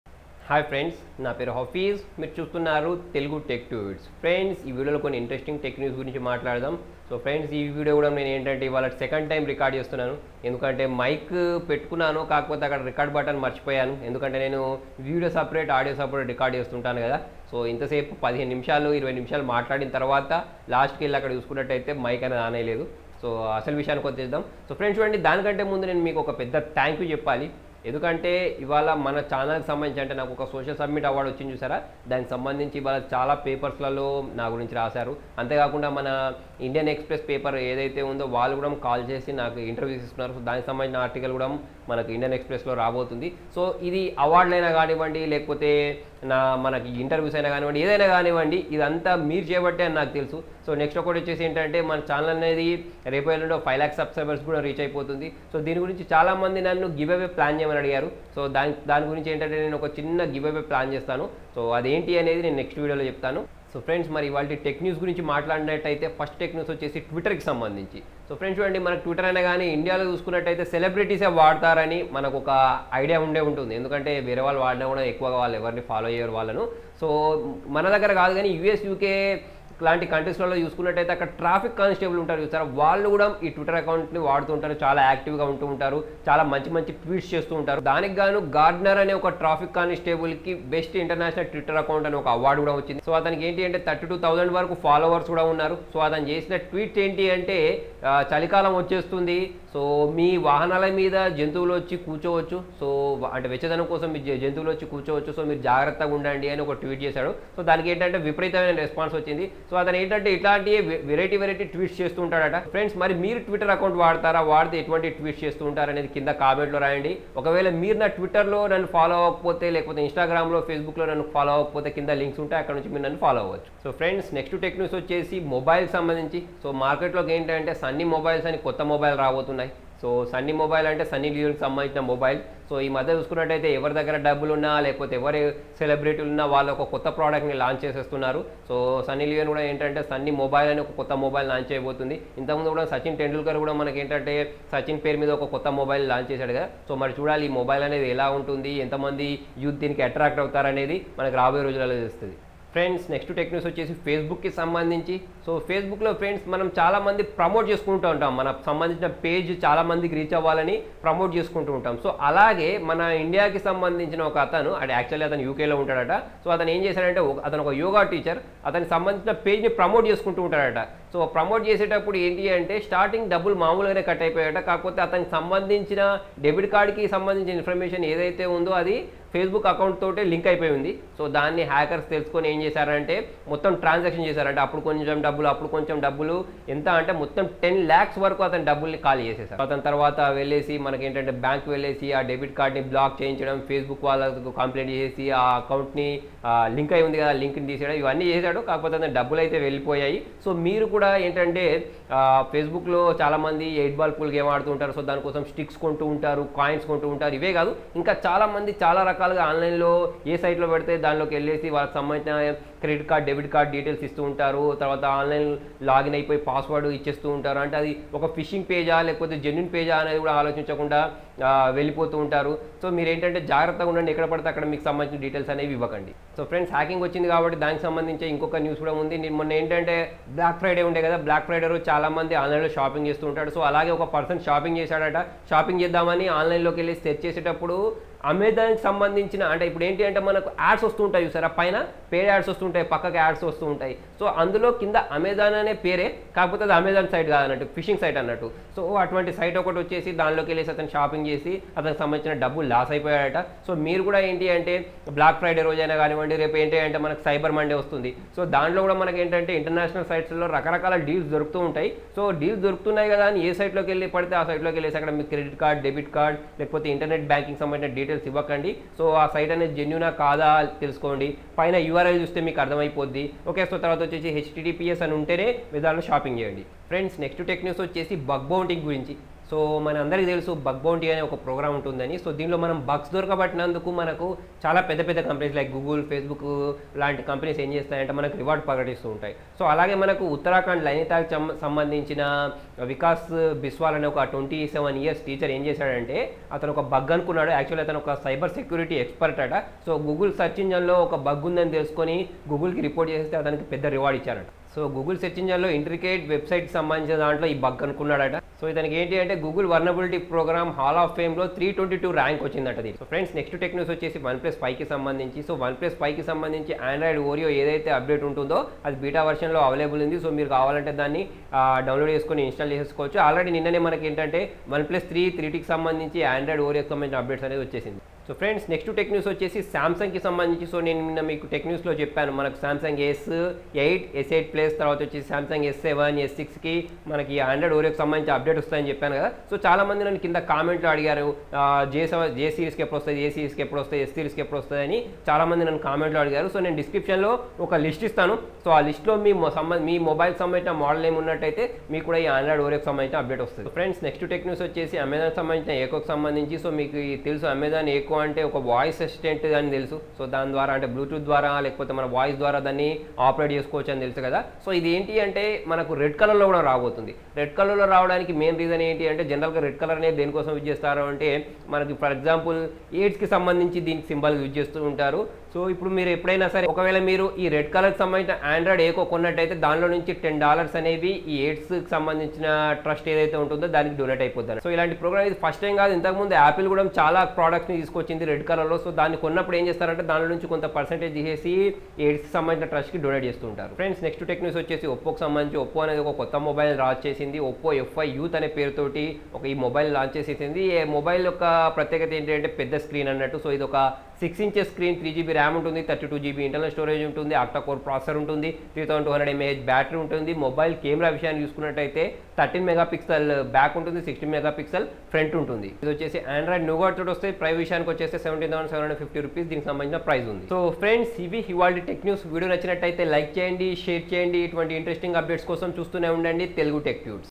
Tech News in Telugu # 38 : Samsung oreo update list ,sunny leones Mobile, Twitter Award
technology videos in telugu : Samsung oreo update list
Galaxy S Series : Galaxy S8, S8+ (Oreo Beta వచ్చింది). Galaxy S7, S7 Edge, S6, S6 Edge, S6 Edge+ Galaxy Note Series : Galaxy Note 8, Note 7 (FE), Galaxy Note 5. Galaxy A Series : Galaxy A7 (2018 Model), Galaxy A5 (2018), Galaxy A3 (2018), Galaxy A7, A5, A3 2017 Model. Galaxy A9 Pro 2016, Galaxy A8 2016. అలాగే OnSeriesలో OnMaxకి Android Oreo వస్తుంది

Galaxy J Series phones: Galaxy J7 2016, J7 Pro, J5 Pro, J7+, J7 Max, J7 Prime, J7 Core. Galaxy C : Galaxy C10, Galaxy C9 Pro, C7 Pro, C5 Pro, C7.

hafiztime
hafiz teugu videos